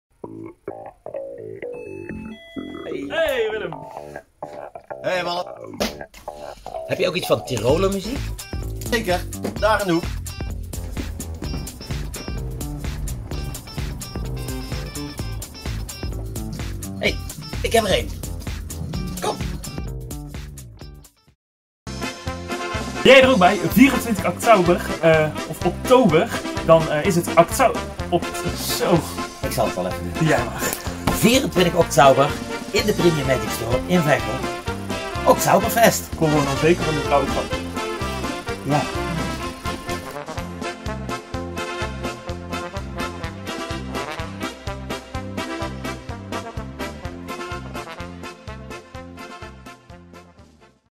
OKTZAUBERFEST 2020 TEASER 2
Magic Mike en Sven Bosch (de Biergoochelaar) gaan alvast op zoek naar de perfecte tirolermuziek voor tijdens het Oktzauberfest.
De beste platenwinkel vind je namelijk ook in Veghel: Platenzaak wil'm